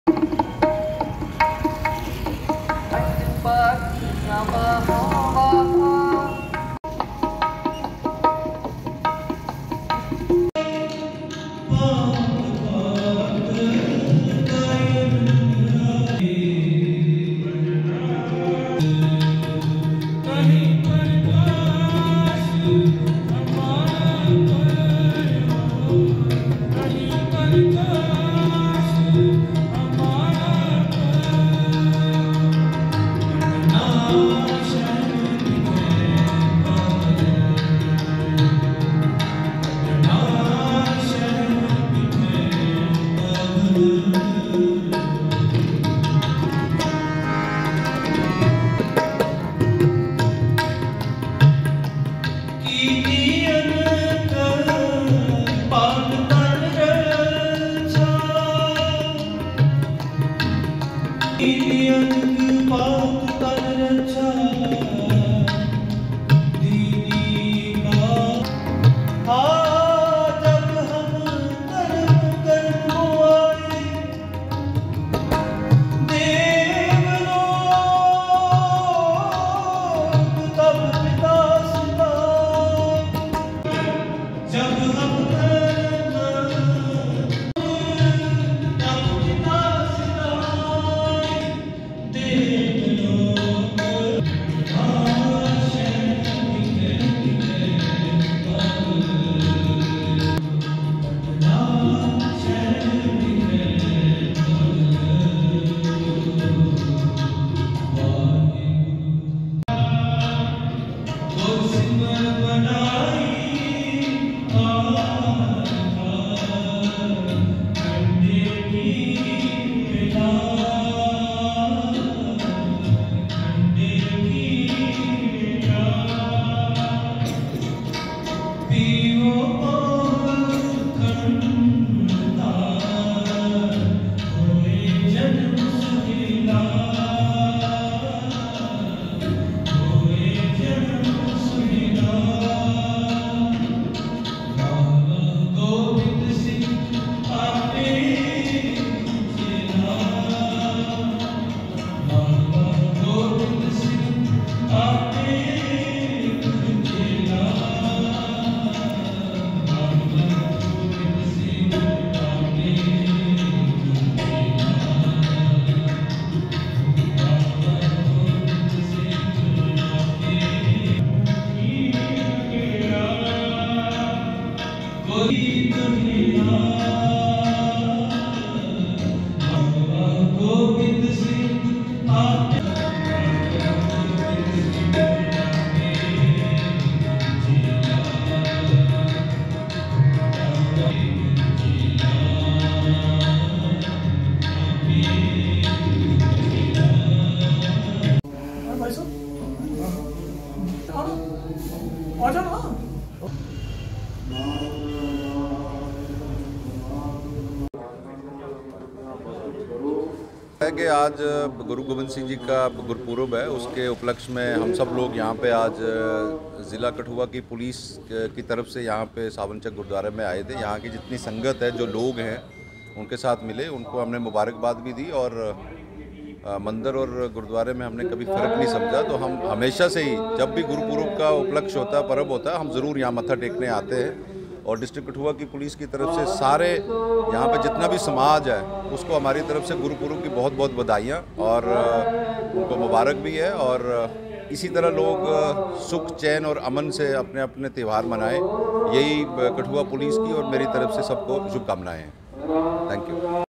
गुरु गोबिंद सिंह जी के प्रकाशोत्सव के उपलक्ष्य पर आज एसएसपी कठुआ आरसी कोतवाल अपने अन्य पुलिस
गुरु गोबिंद सिंह जी के प्रकाशोत्सव के उपलक्ष्य पर आज एसएसपी कठुआ आरसी कोतवाल अपने अन्य पुलिस अधिकारियों के साथ गुरुद्वारा सिंह सभा सावन चक में माथा टेकने पहुंचे और साथ ही उन्होंने सभी शहरवासियों को गुरुपूर्व की बधाई भी दी।